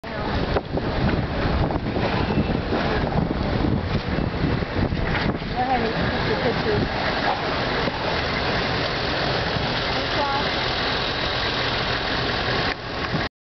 Devastation in Haiti (Church)
This is just a quick video of a devestated church in Port au Prince, Haiti. I am sorry for the low quality in the video but you basicly get how bad Haiti was struck. It is not as bad as Japan though.